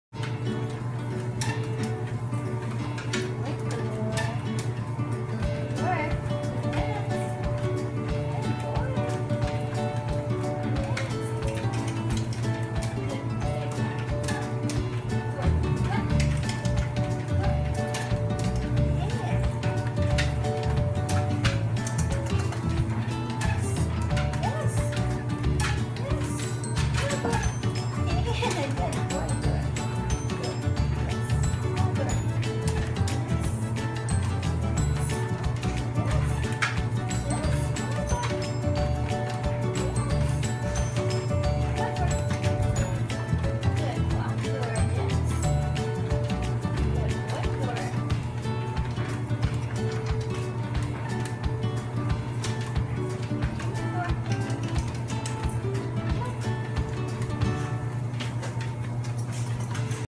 Training Husky To Walk on Treadmill
Thor, our siberian husky show dog getting some lessons in the treadmill. This is part of his training on his journey to championship!